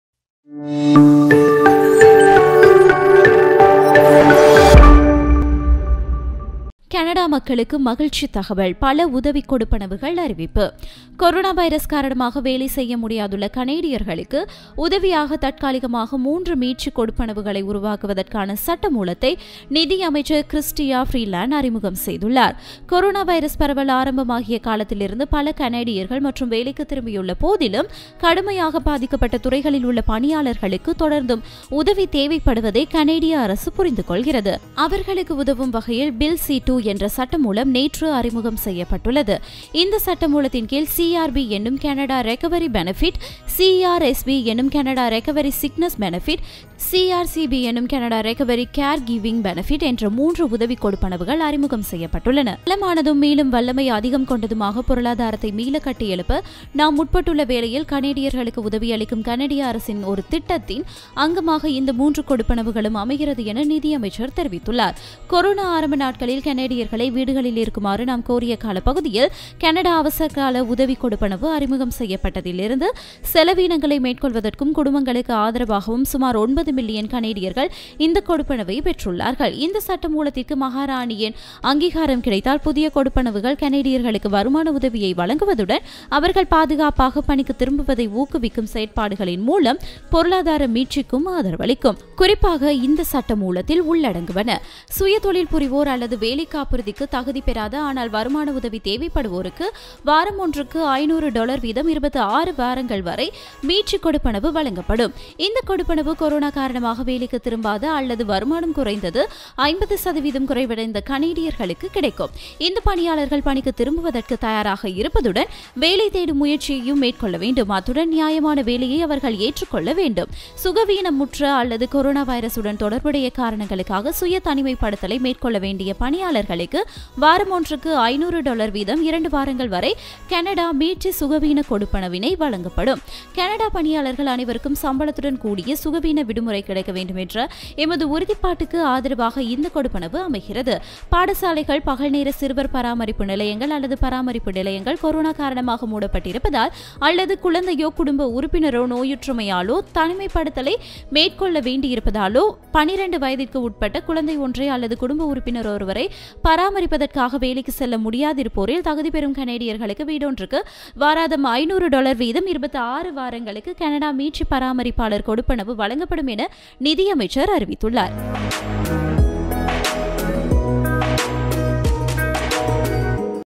கனடா மக்களுக்கு மகிழ்சித் தகவல்! பல உதவித் கொடுப்பனவுகள் அறிவிப்பு
கொரோனா வைரஸ் காரணமாக வேலை செய்ய முடியாதுள்ள கனேடியர்களுக்கு உதவியாகத்  தற்காலிகமாக மூன்று மீட்சிக் கொடுப்பனவுகளை உருவாக்குவதற்கான சட்டமூலத்தை நிதியமைச்சர் கிரிஸ்டியா பீரிலேண்ட் (Chrystia Freeland) அறிமுகம் செய்துள்ளார்.

சுய தொழில் புரிவோர் அல்லது வேலைக்காப்புறுதிக்குத் தகுதி பெறாத, ஆனால் வருமான உதவி தேவைப்படுவோருக்கு வாரமொன்றுக்கு 500 டொலர் வீதம் 26 வாரங்கள் வரை மீட்சிக் கொடுப்பனவு வழங்கப்படும். இந்தக் கொடுப்பனவு, கொரோனா காரணமாக வேலைக்குத் திரும்பாத அல்லது வருமானம் குறைந்தது 50 சதவீதம் குறைவடைந்த கனடியர்களுக்குக் கிடைக்கும்.